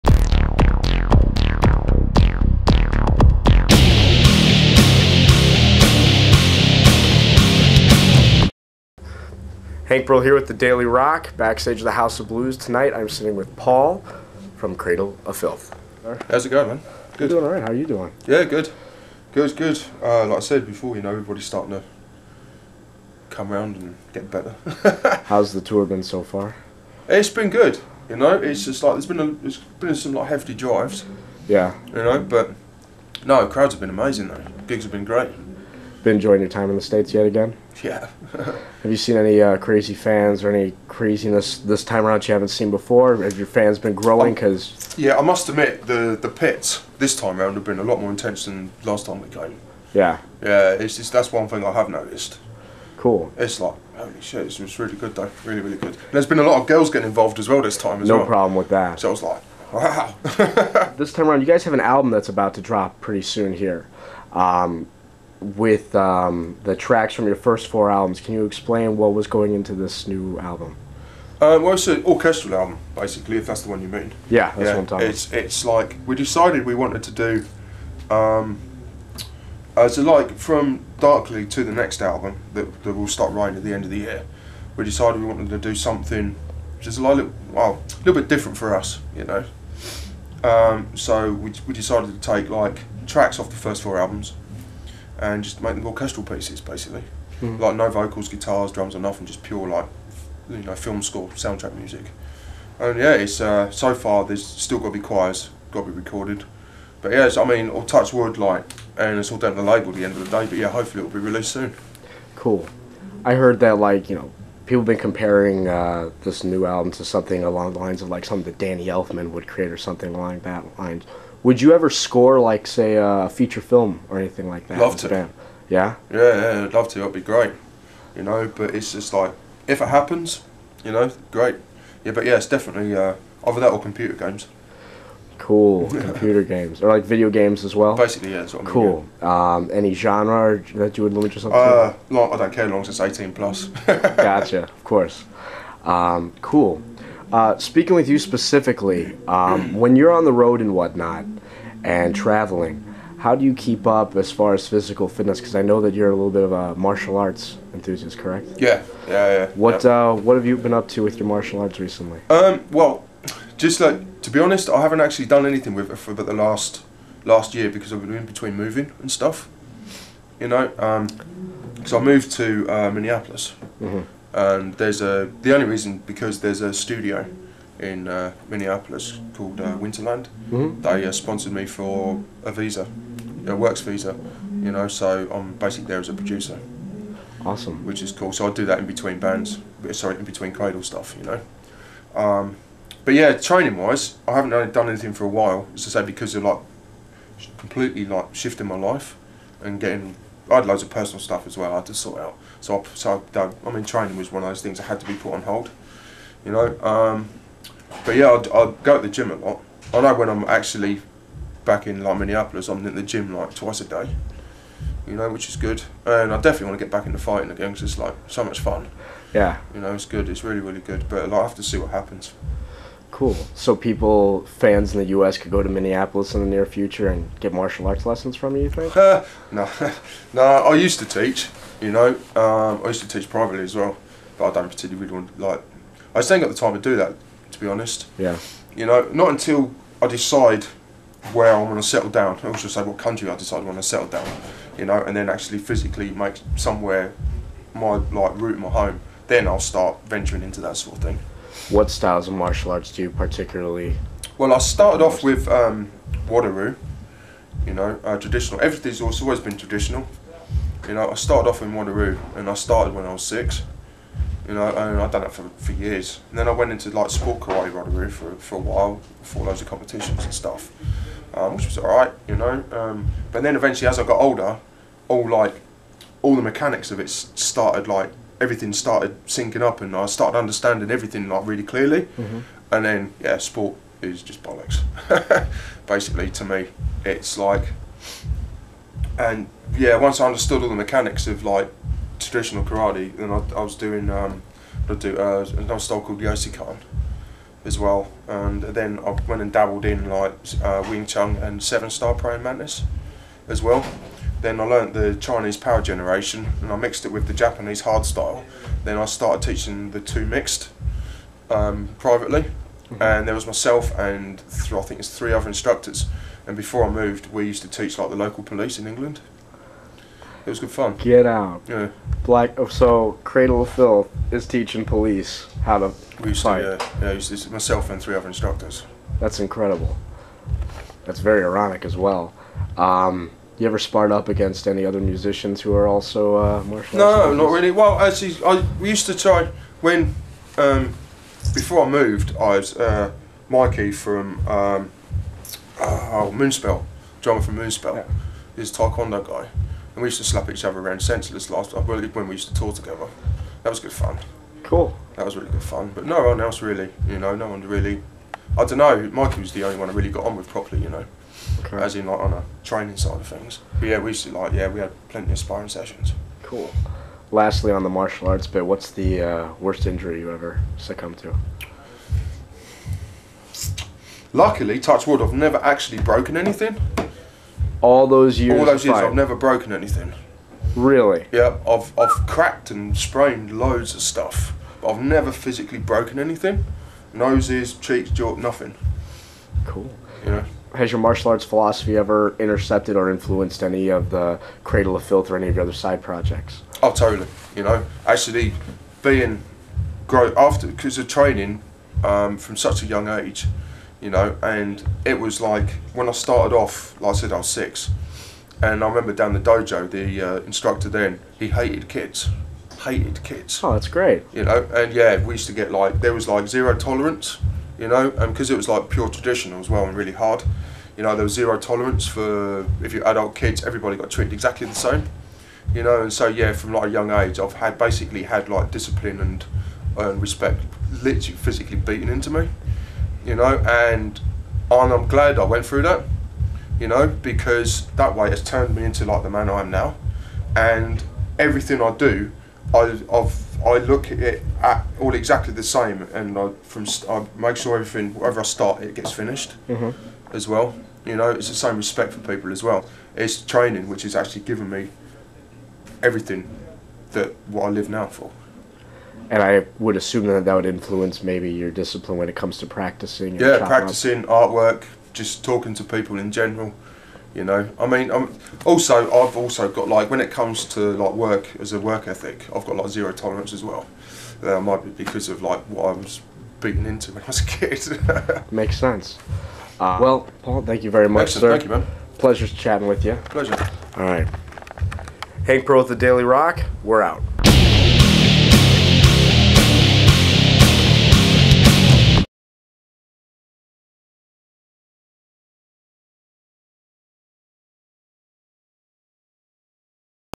Cradle of Filth Interview with Paul Allender
The Daily Rock's Hank Pearl interviews Paul Allender of British Extreme Metal group CRADLE OF FILTH before a performance at the House of Blues Chicago.

After touching base on the state of the tour and the increasingly gruesome pits at the shows, Allender discusses in depth his extensive martial arts conditioning, his history of training British police in hand-to-hand combat, etc., and how it all has come to influence his mind and method of creating some of CRADLE OF FILTH's musical stylings.